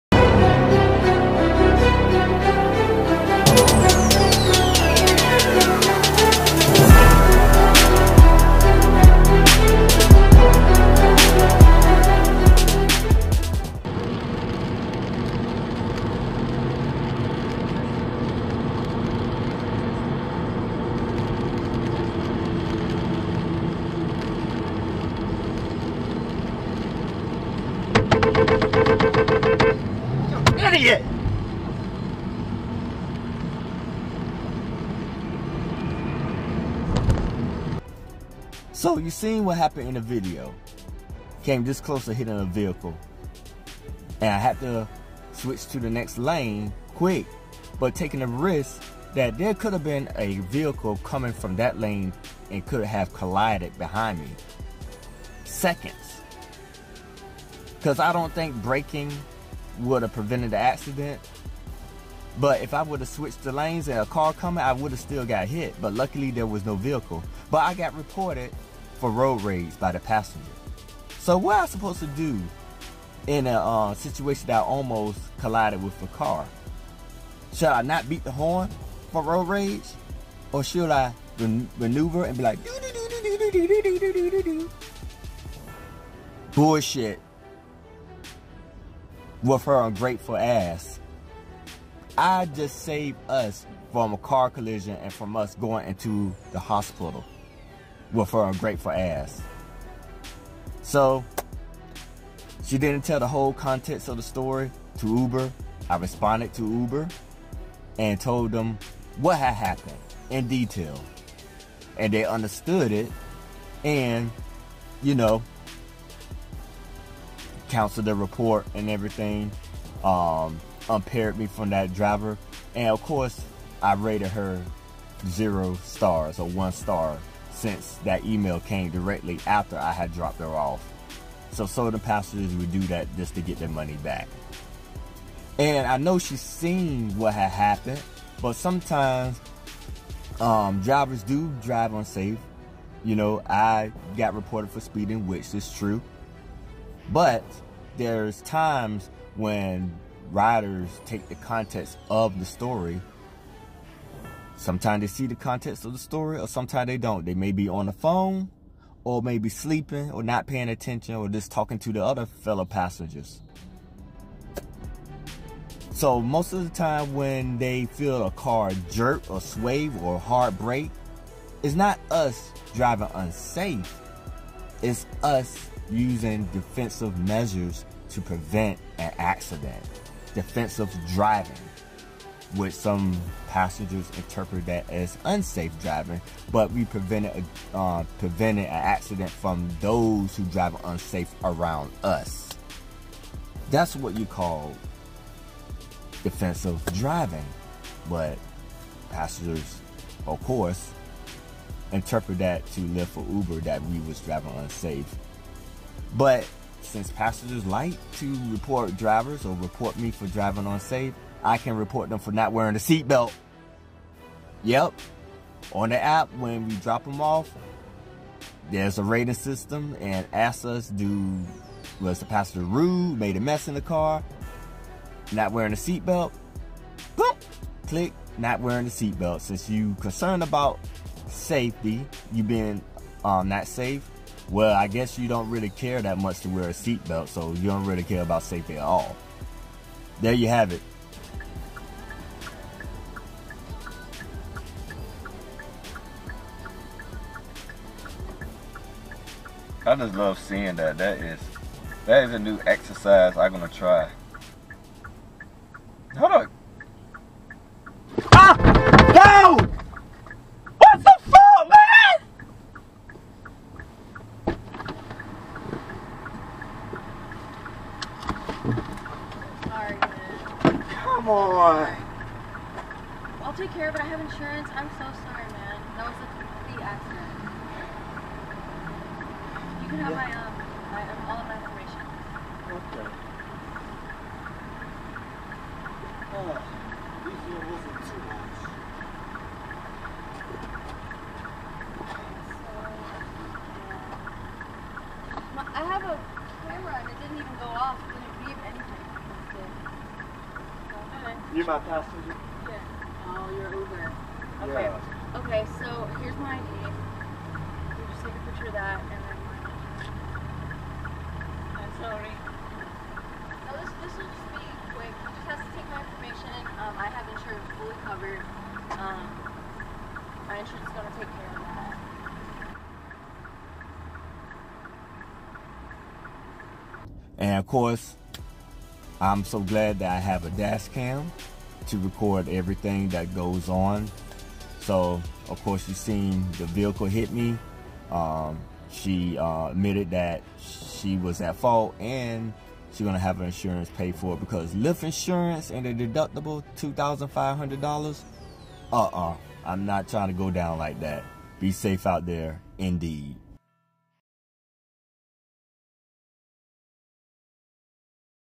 Uber Driver Gets Reported for Road Rage
Sometimes riders text the story out of context just to get their money back.